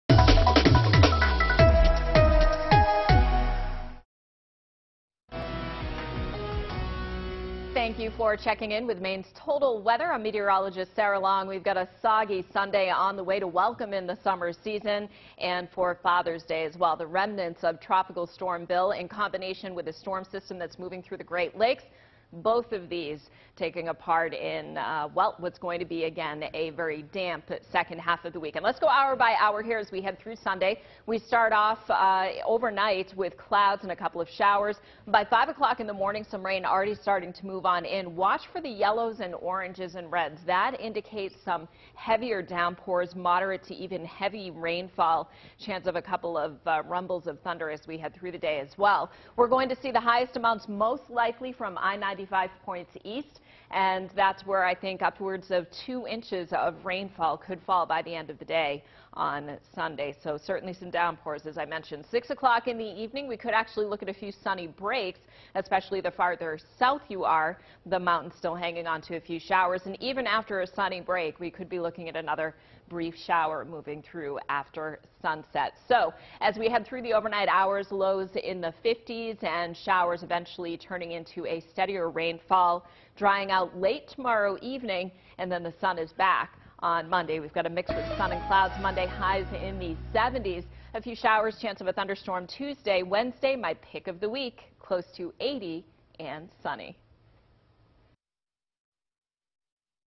Sarah’s Saturday evening forecast
How’s the weather looking for your weekend? Get your latest Maine’s Total Weather video forecast from Meteorologist Sarah Long.